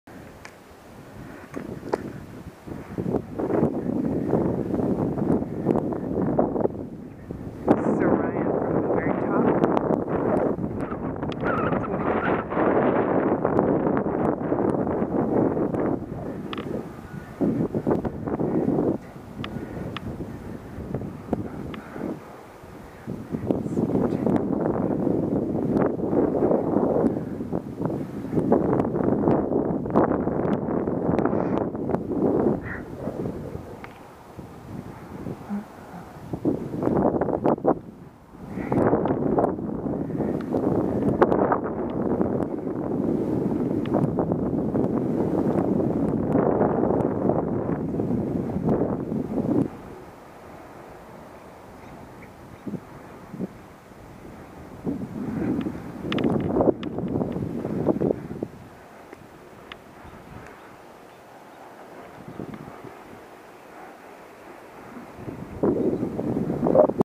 Pullman, One Street at a Time, Orion, July 28, 2013
Unicycling down Orion on Military Hill in Pullman, Washington.